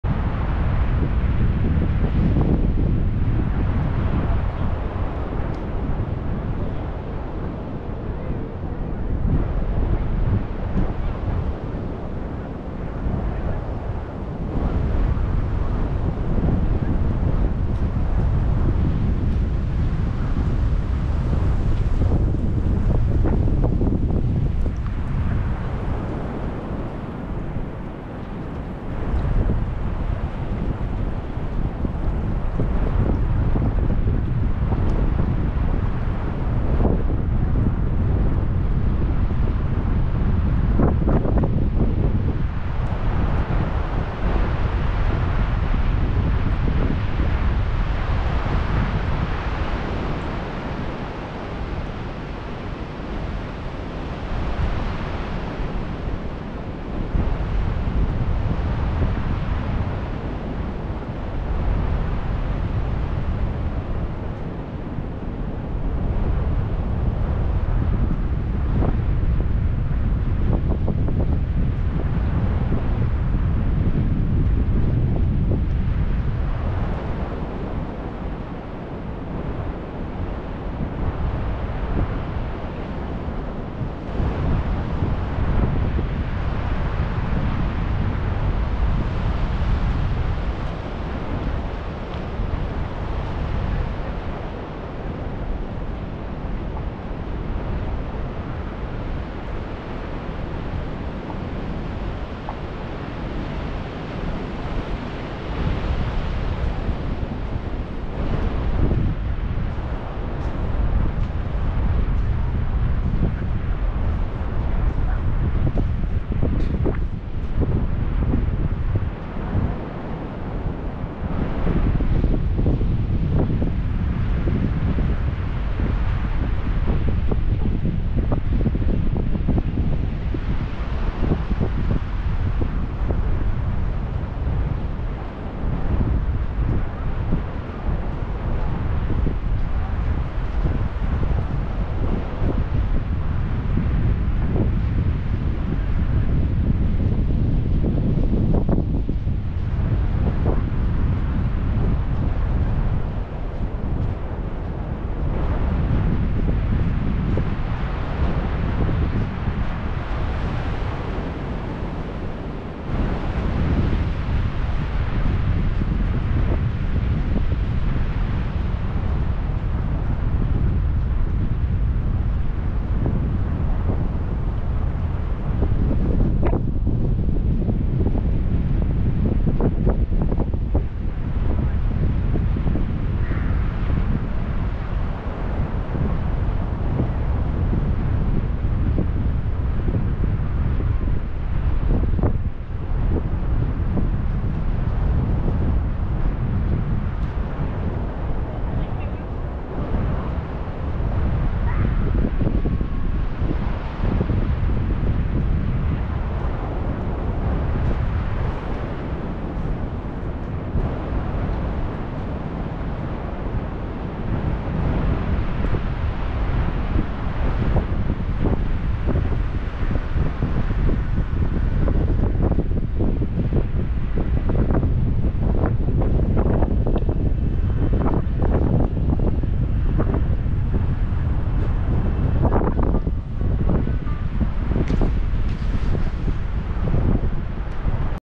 Cannon Beach - Haystack Rock - Ebb & Flow II
Saturday, January 20, 2024, at Cannon Beach next to Haystack Rock absorbing the ebb & flow